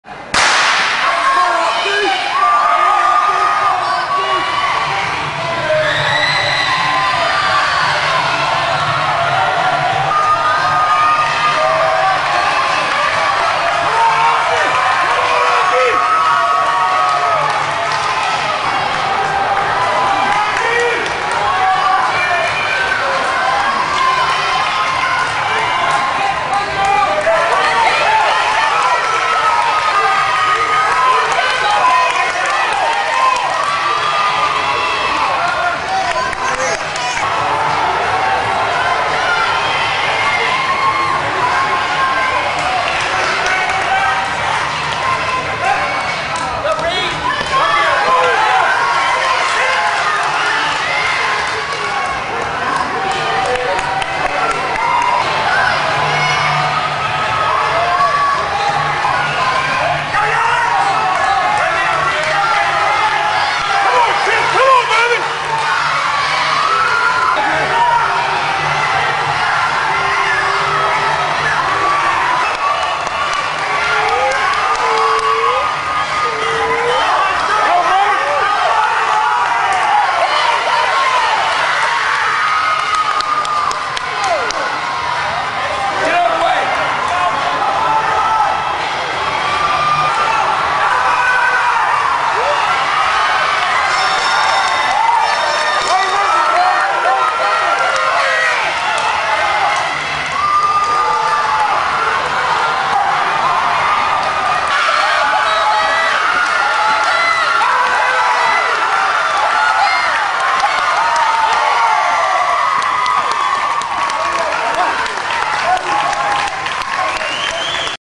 2011 VISAA Indoor State Championships 4x400m Final
#1 Woodberry Forest-3:30.35
#2 St. Christopher's-3:31.44
#3 Episcopal High School-3:32.16
#4 Fork Union Military Academy-3:36.96